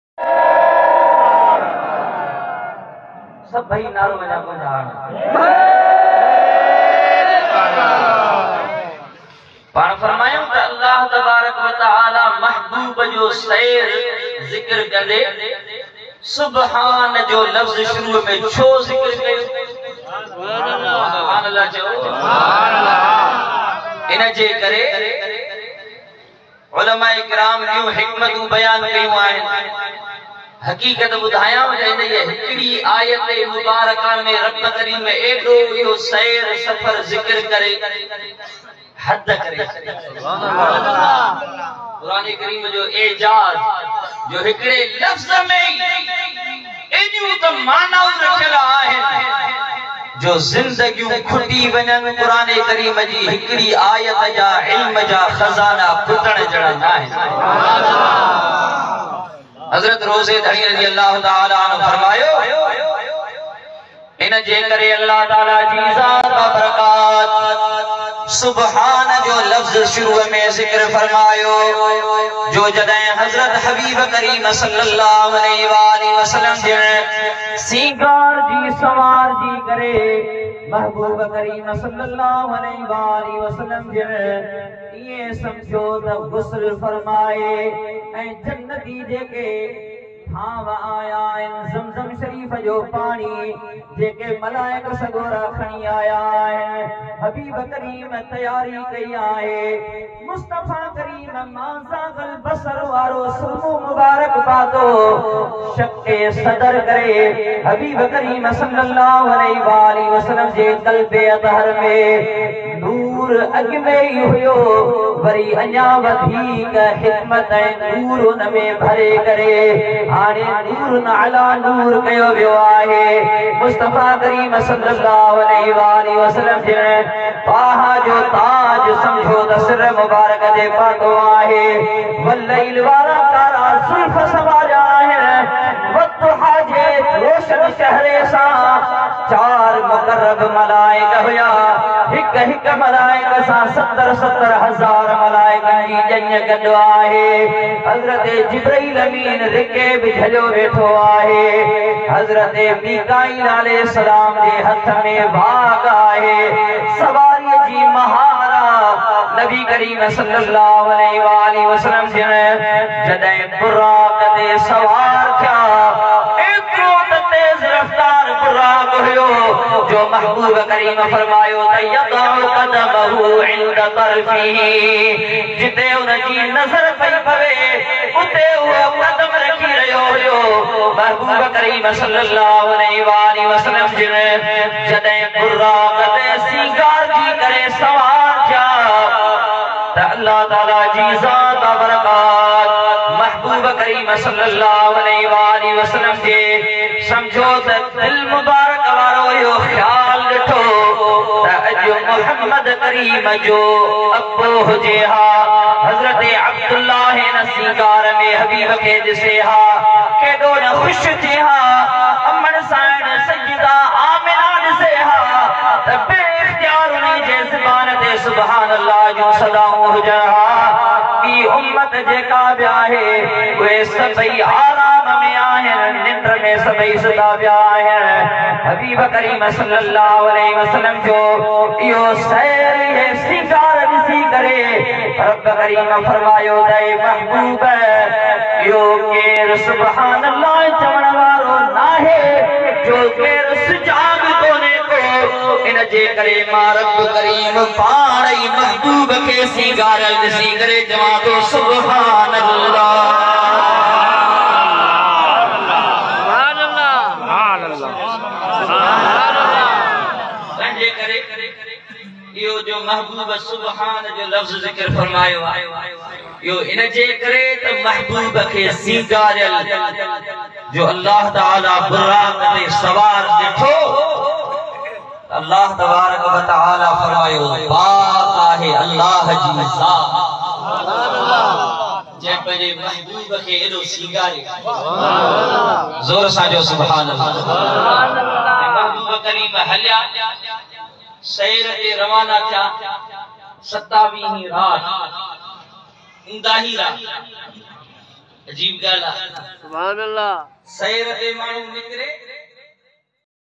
Shab e Meraj 27 Rajab Mufti Inayatullah Sikandari At Dargah Sharif Pir Jo Goth
Shab e Meraj 27 Rajab
Mufti Inayatullah Sikandari
At Dargah Sharif Pir Jo Goth

"شب معراج"
"27 رجب جي رات"
شير روضي ڌڻي مفتي عنايت الله سڪندري نيو بيان
درگاھ شريف پير جو ڳوٺ
بهترين بيان ٻڌو ۽ شير ڪريو
.مهرباني ڪري اسان جي هن چينل کي سبسڪرايب ڪريو